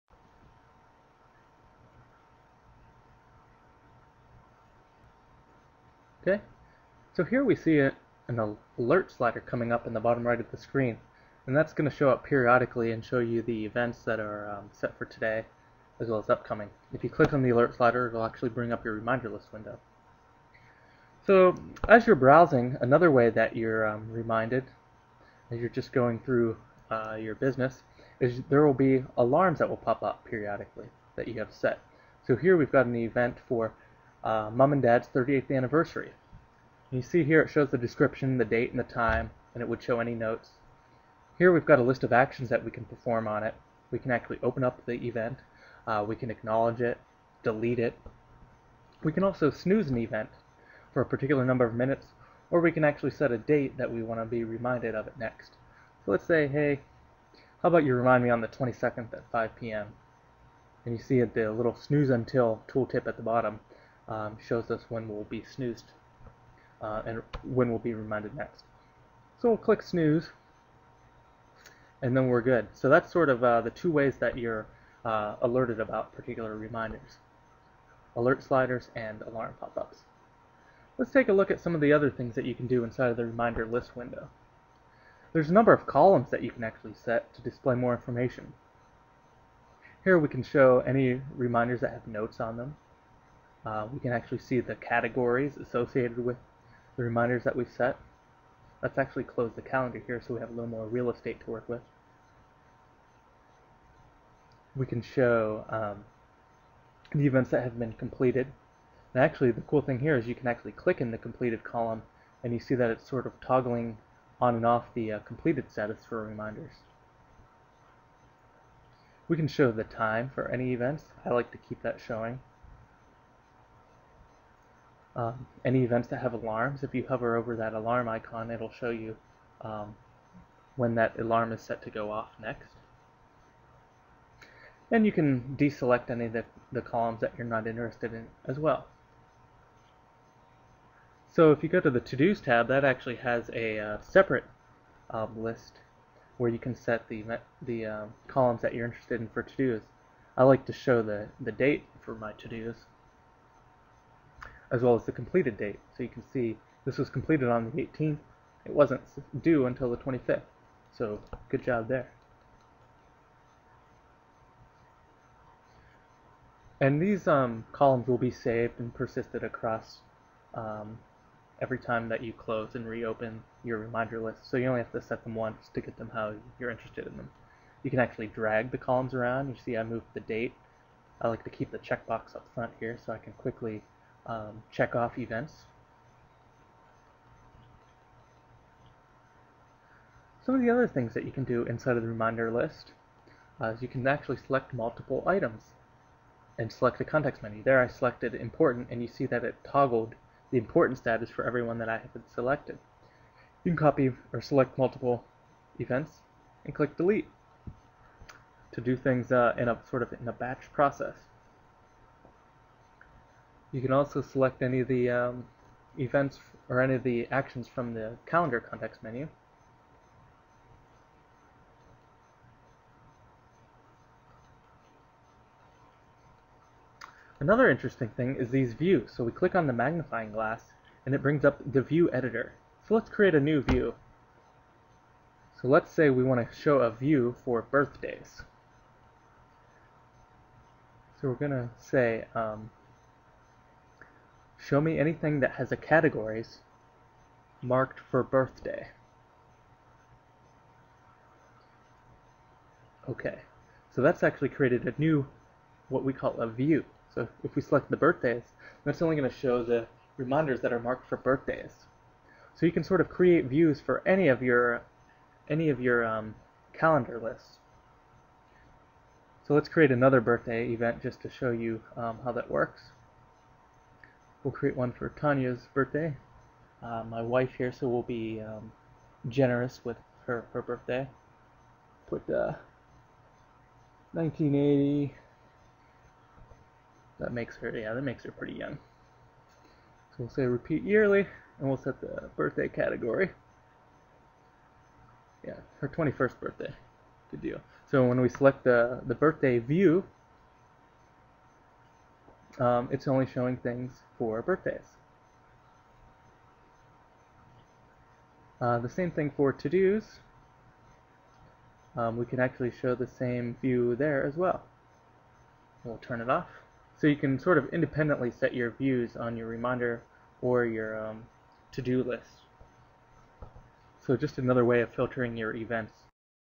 ReminderFox 3 - Alerts, Alarms, Views
Learn about how ReminderFox alerts you via alert sliders and alarms. See how you can add new list columns, and learn how you can add and use Views to see just the reminders you want.